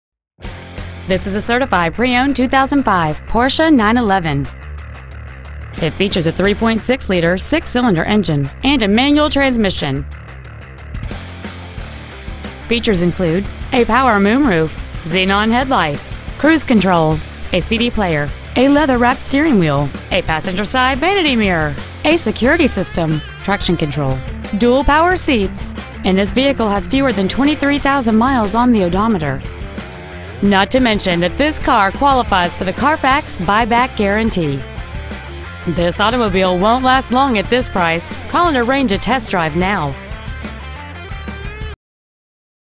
Certified 2005 Porsche 911 Carrera Houston TX 77074
Year : 2005
 Make : Porsche
 Model : 911 Carrera
 Engine : 3.6L H-6 cyl
 Trans . : Manual
 Exterior : Atlas Grey Metallic
 Miles : 22,904
 Interior : Platinum Grey

 Pre-Owned 2005 Porsche 911 Carrera Houston TX